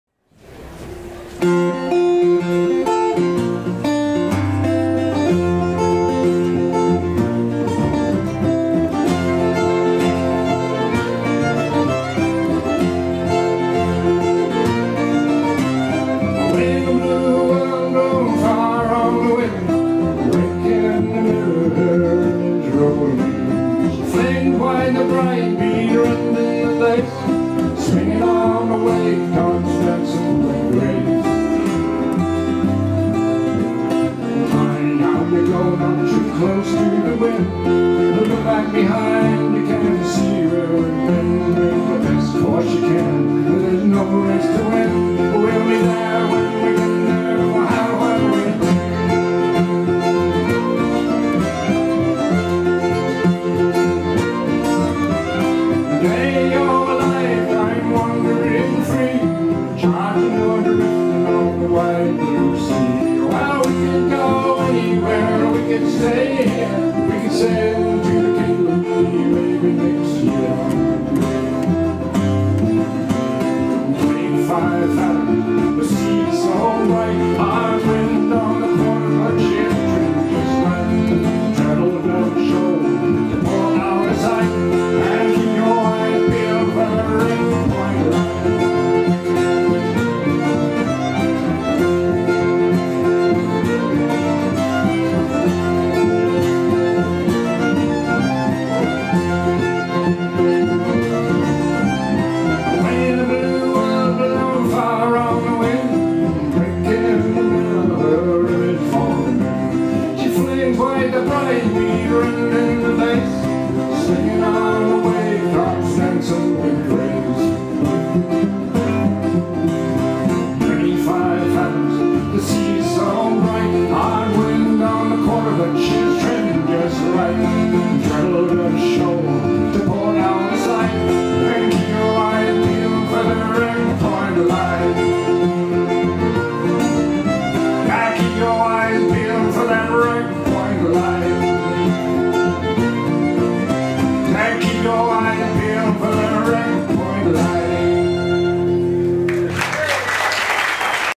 The Sideshow Allies band playing at fairbridge festival 2016 7592
Rick Martin’s rich voice and moving songs will be remembered by fans of Bungarra at the Seaview Hotel in Fremantle in the 1980’s. Peter Grayling (cello, mandola, vocals) Rachel Armstrong (violin, ukulele, mandolin, vocals) and Gary Burke (double bass, ukulele, vocals) fill out the musical palette with melody, rhythm and vocal harmony, and contribute songs of their own.
Western Australia